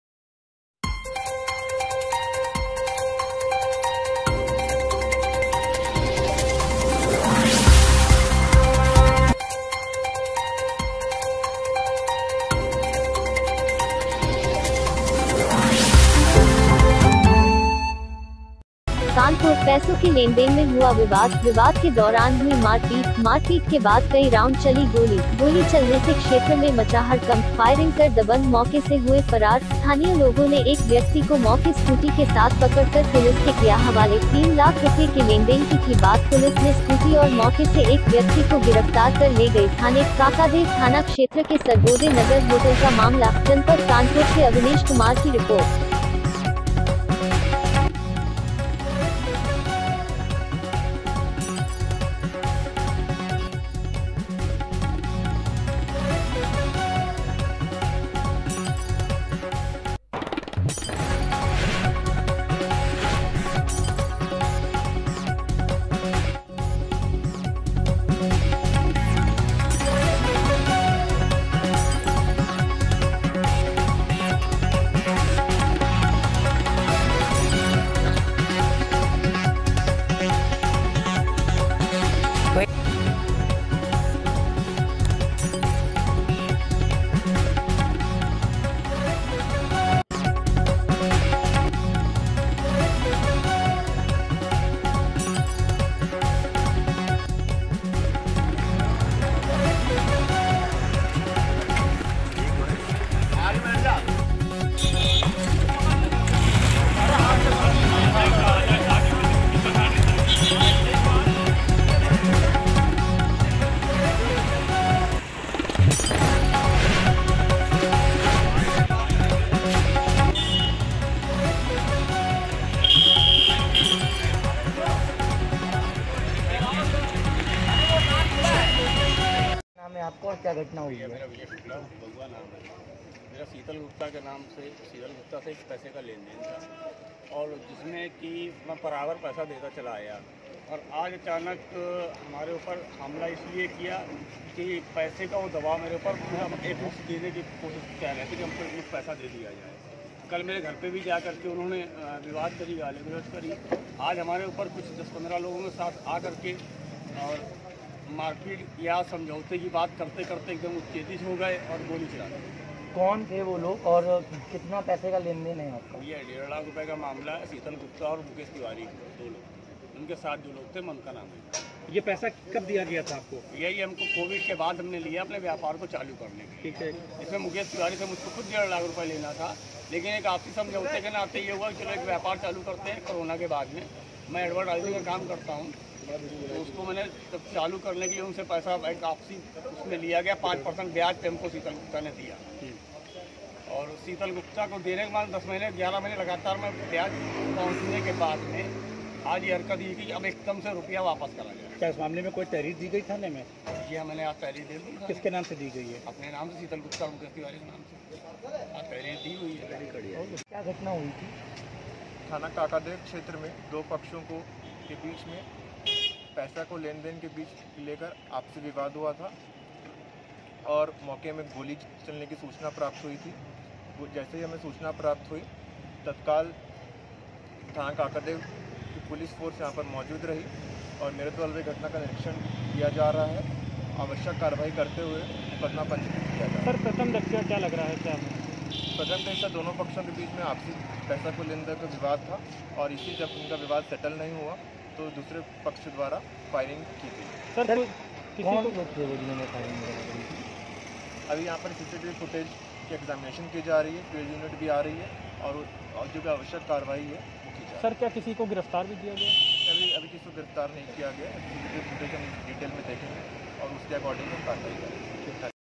कानपुर में पैसों के लेनदेन में गोली चलाकर दबंग मौके से हुए फरार
कानपुर--पैसों के लेनदेन में हुआ विवाद।

विवाद के दौरान हुई मारपीट।

मारपीट के बाद कई राउंड चली गोली ।

पैसों के लेनदेन में गोली चलने से क्षेत्र में मचा हड़कंप ।

फायरिंग कर दबंग मौके से हुए फरार ।

स्थानीय लोगो ने एक व्यक्ति को मौके स्कूटी के साथ पकड़कर पुलिस के किया हवाले ।

3 लाख रुपये के लेनदेन की थी बात ।

पुलिस ने स्कूटी और मौके से एक व्यक्ति को गिरफ्तार कर ले गई थाने ।

काकादेव थाना क्षेत्र के सर्वोदय नगर होटल का मामला ।